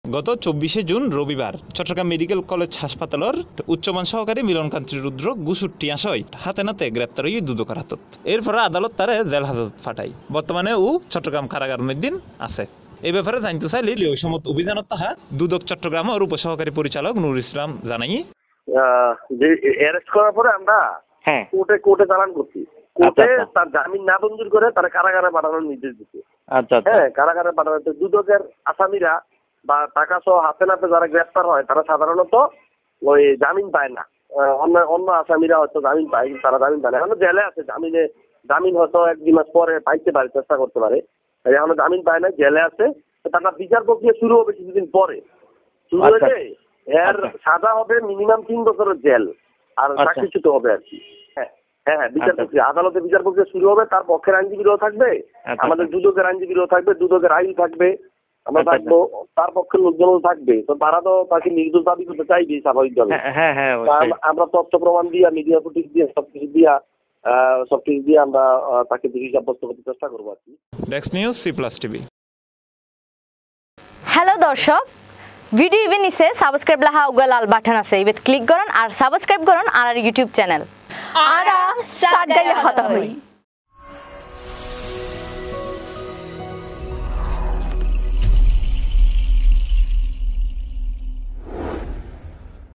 ফলোআপ: গত ২৪ জুন রবিবার দুদকের হাতে ঘুষের টাকাসহ গ্রেফতার কর্মচারী এখনো কারাগারে
গত ২৪ জুন রবিবার দুদকের হাতে ঘুষের টাকাসহ গ্রেফতার চট্টগ্রাম মেডিকেলের কর্মচারী মিলন কান্তি রুদ্র এখনো কারাগারে আছেন। এই বিষয়টি নিয়ে আমরা কথা বলেছিলাম দুদকের উপ সহকারী পরিচালকের সাথে